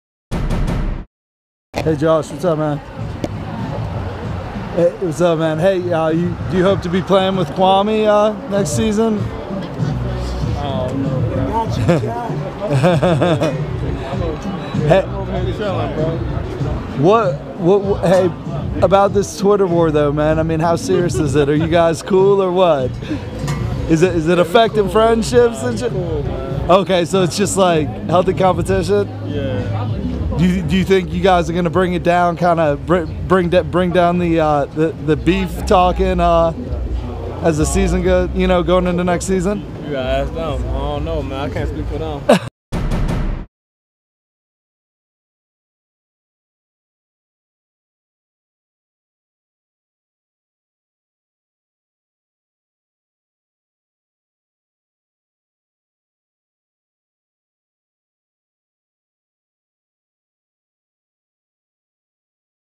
Lakers' Josh Hart Says Lonzo:Kuzma Twitter Beef Ain't A Big Deal | TMZ Sports
Anyone waiting for Kyle Kuzma and Lonzo Ball to get into a full blown beef is about to be disappointed ... because Lakers guard Josh Hart says everything is cool between the two guys.